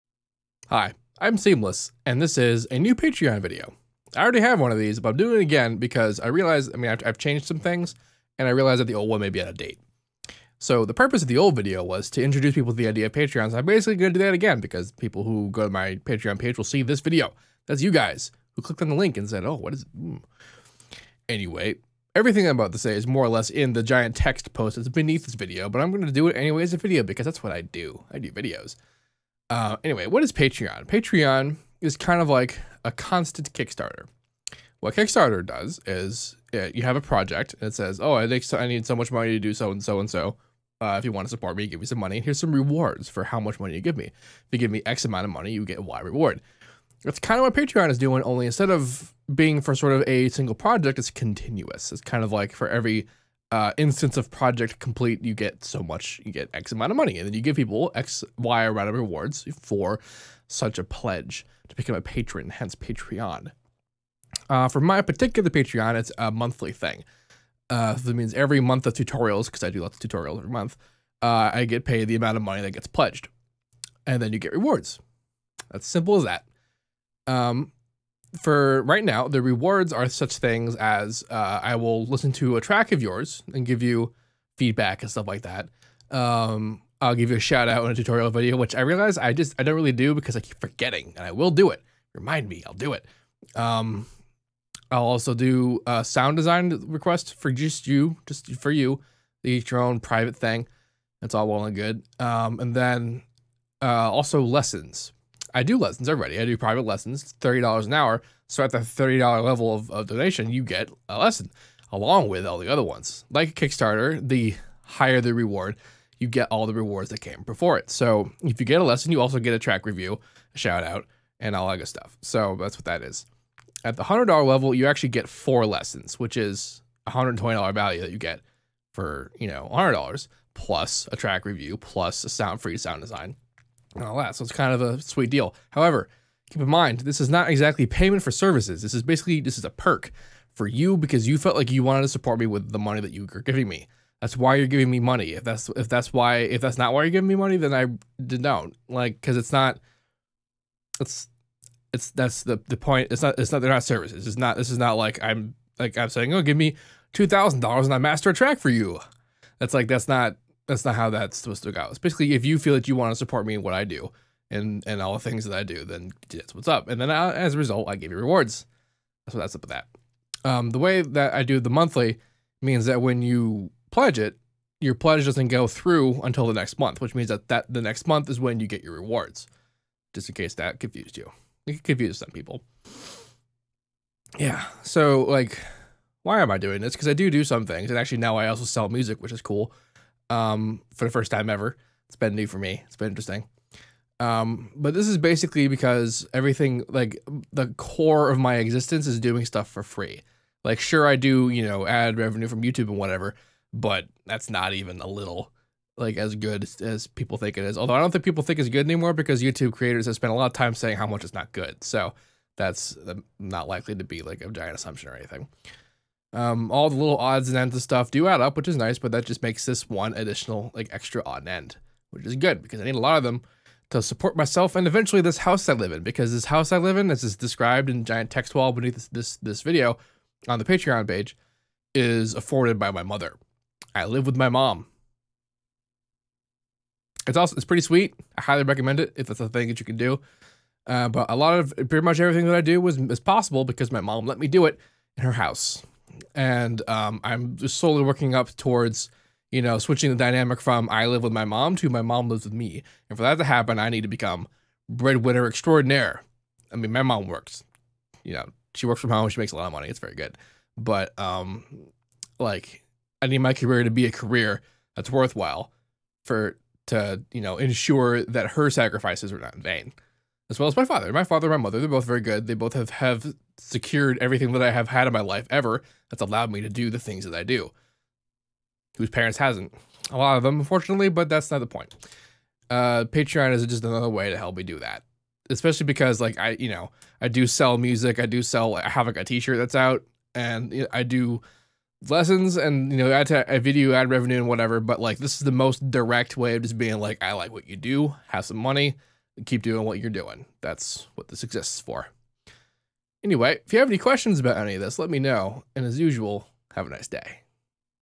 SeamlessR on Patreon (2014 Update)
I'm doing this video to replace the old "explanation video" that's up on the Patreon page. Current information :)